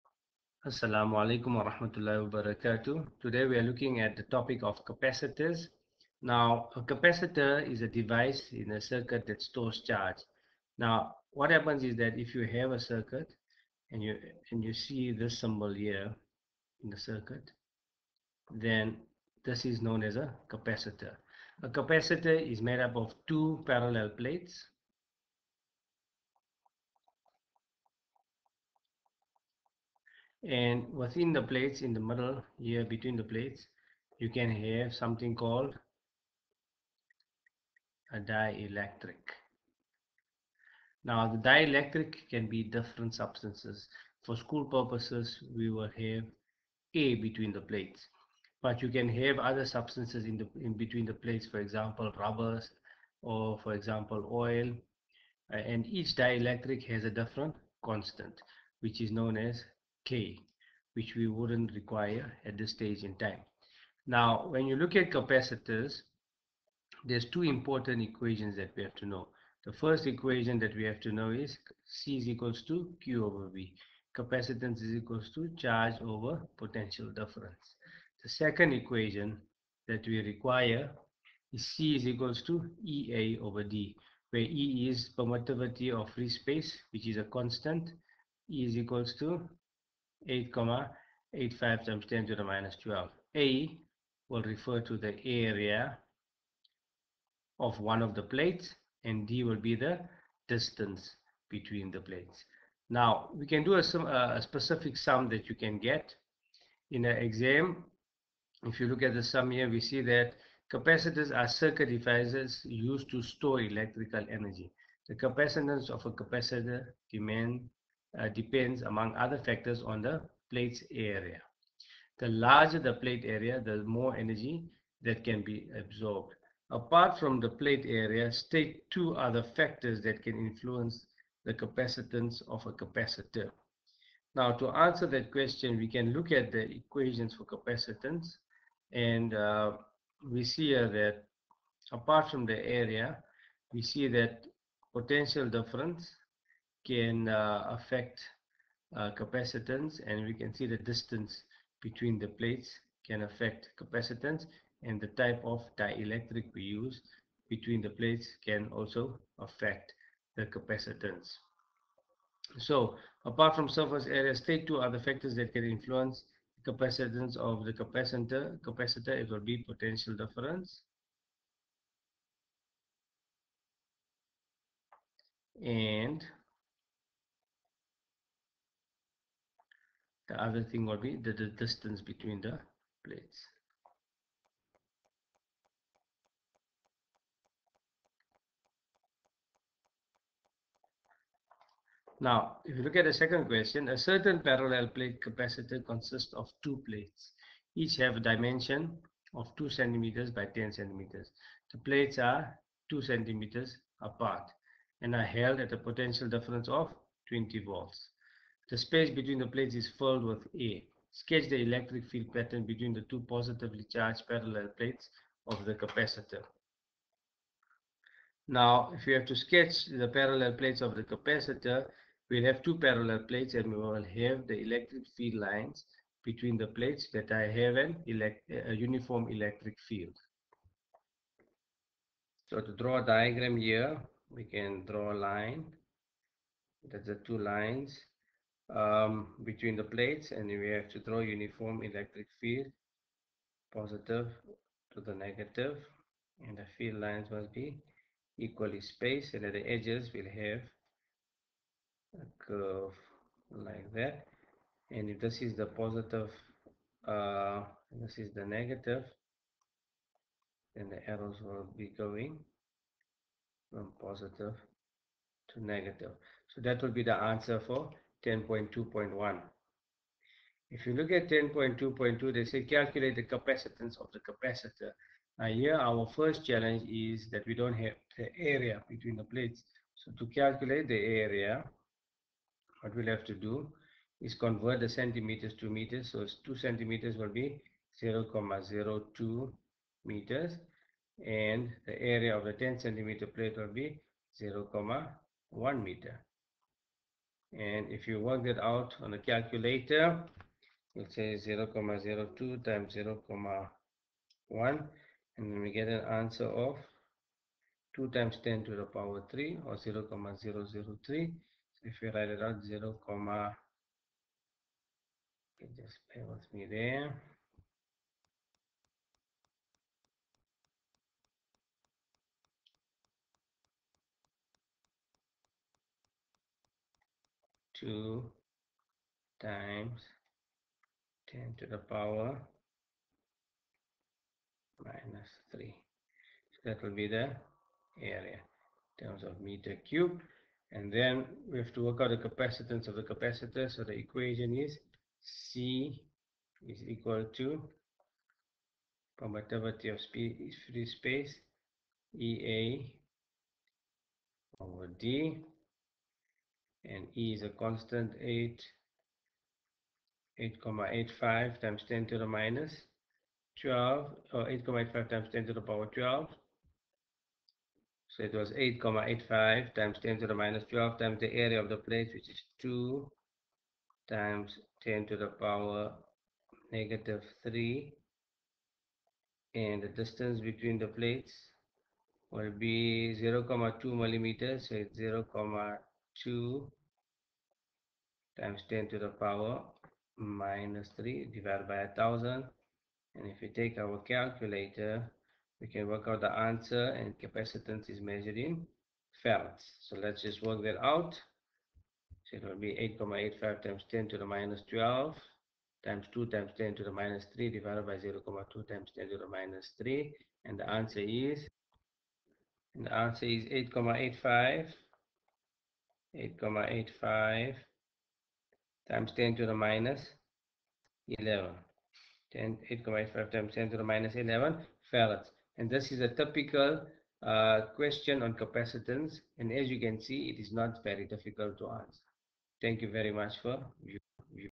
Capacitors with exam question
An explanation of the basics of capacitors, with a typical exam question answered.